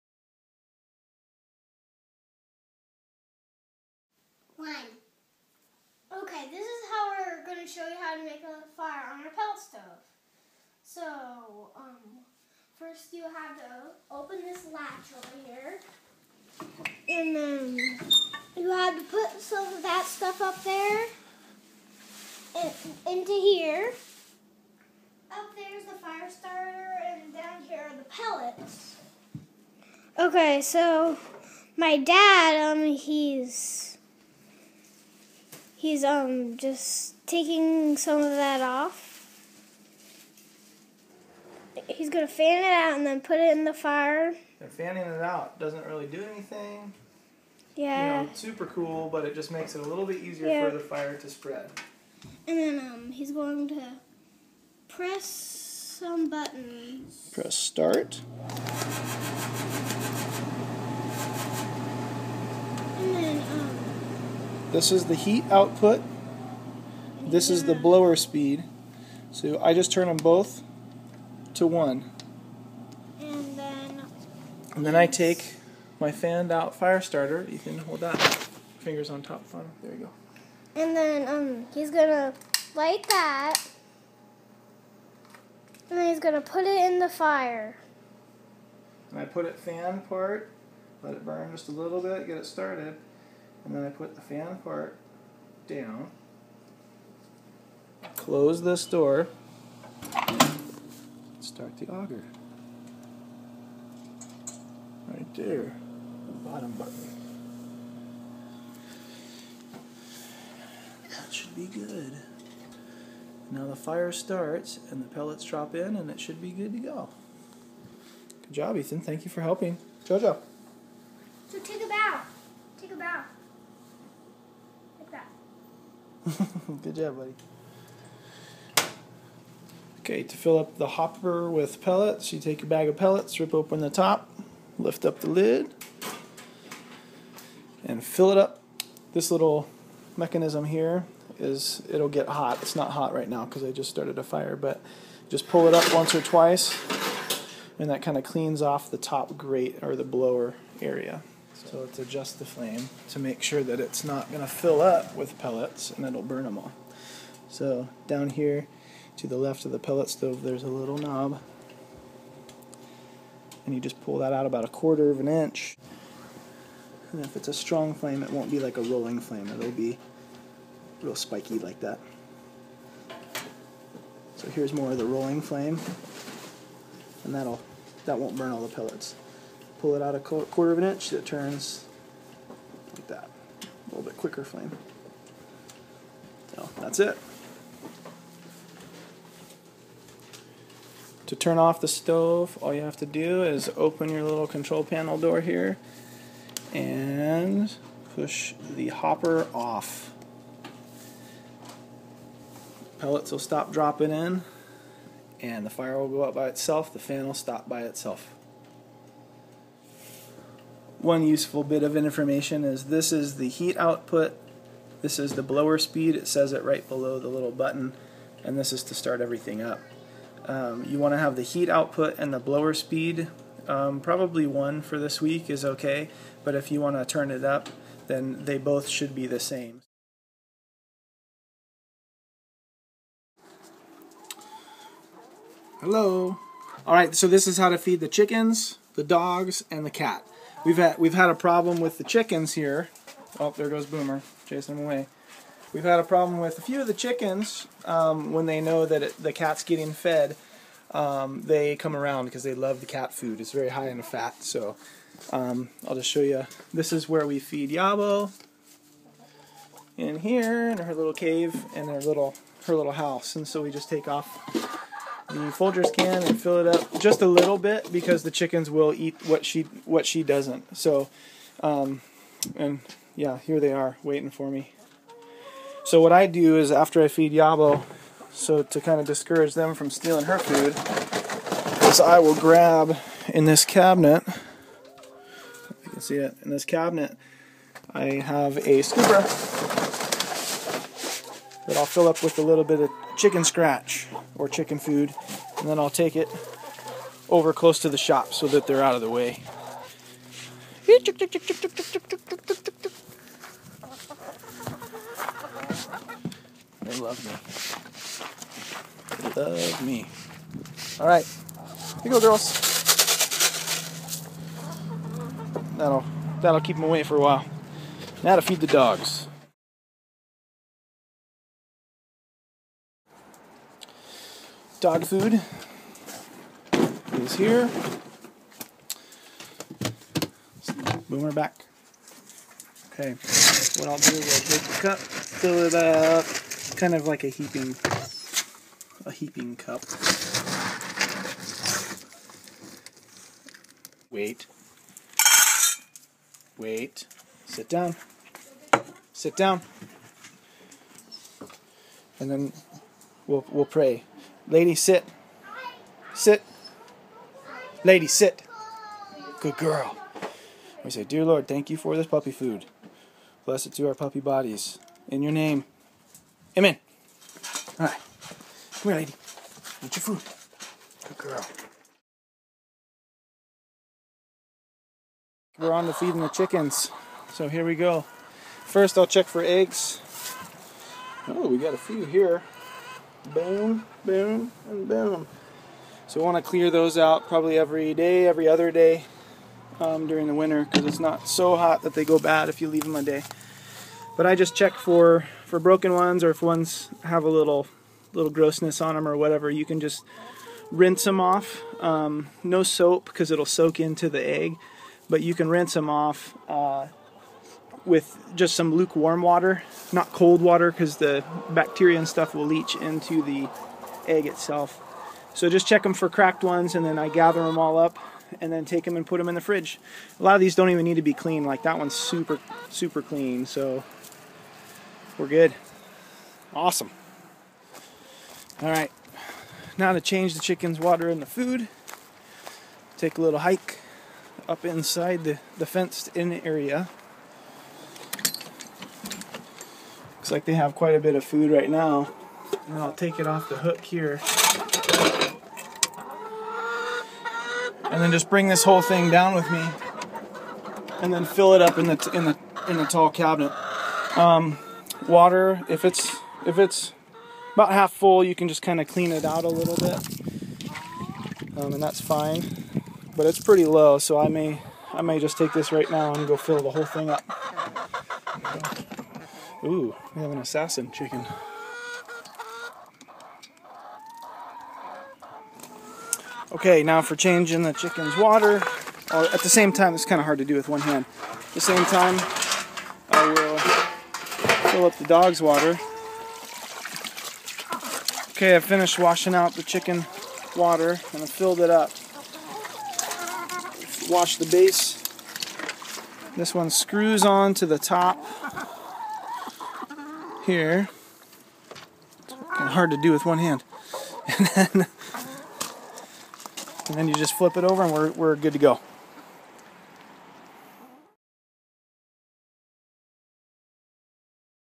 Pellet stove and taking care of animals
Welcome to Solid Rock Ranch!
In this video:
The boys and I, explain how to use the pellet stove.
I also explain (to my most excellent family) how to take care of the animals while we are away.